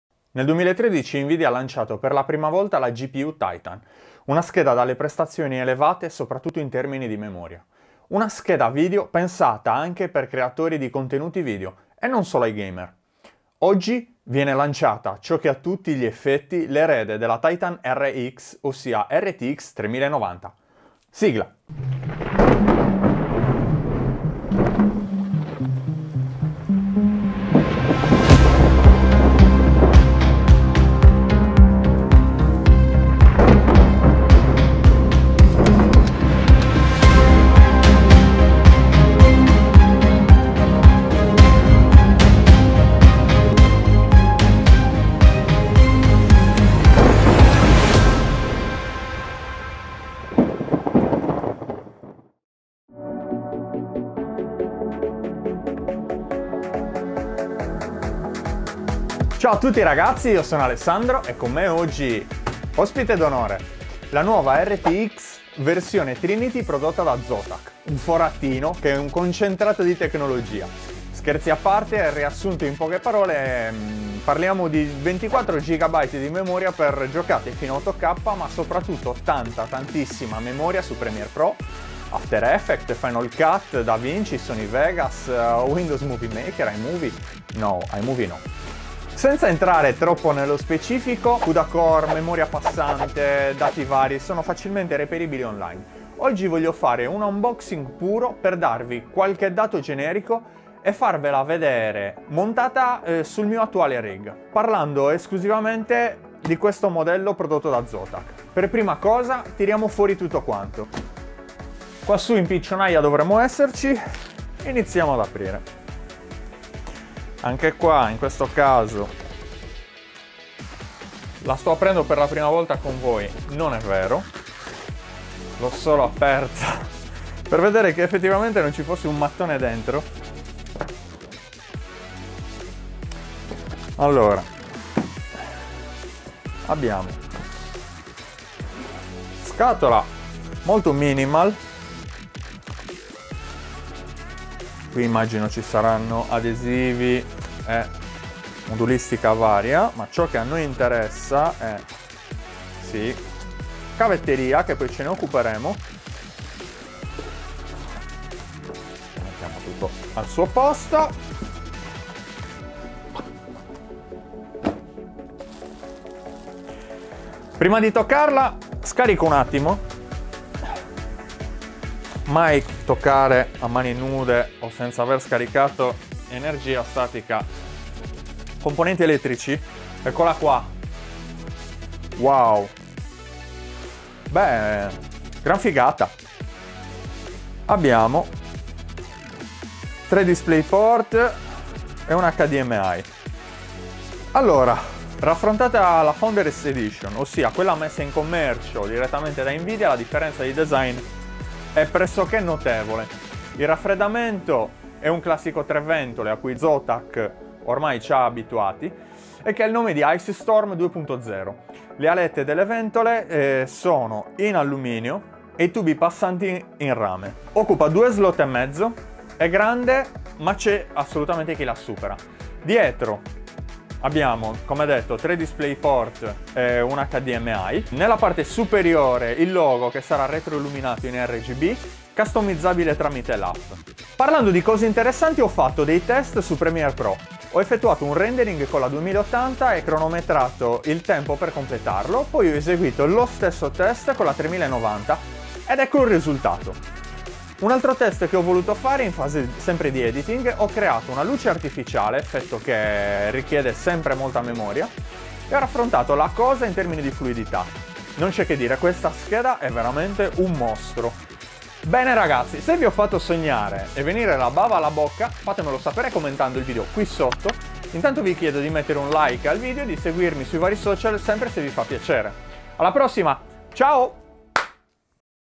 NVIDIA GEFORCE RTX 3090 | Unboxing
Scopriamo la nuova e ricercata 3090 RTX della Nvidia. Giocate a 8k e Rendering da paura con una memoria da 24GB.
Ho fatto un piccolo Unboxing e un semplicissimo test rendering. Per quest'ultimo ho comparato i tempi di rendering contro la precedente scheda (2080) guadagnando quasi 1 minuto e mezzo su una timeline di circa 5 minuti, nella quale ho volutamente inserito due effetti luce volumetrica, così da appesantire decisamente il processo di esportazione.
Se rapportiamo questo minuto e mezzo su molti più effetti e su timeline più complesse, il guadagno di tempo sarà ancora più marcato.
Il miglioramento maggiore l'ho notato sulla timeline, in fase di scorrimento e in fase di preview rendering. Inoltre ho notato che l'intervento della scheda video ha alleggerito e di molto il carico della CPU, permettendogli di lavorare a temperature leggermente inferiori.

00:00 Intro
00:21 Sigla
00:53 RTX 3090
01:51 Unboxing
03:53 Test
04:22 Saluti

Attrezzatura usata:
-Sony A7sIII
-Sony A7sII
-58mm Zenith
-35mm GM 1.4
-Godox SL 60w
-Yongnuo Led
-Manfrotto Led
-Rode Wireless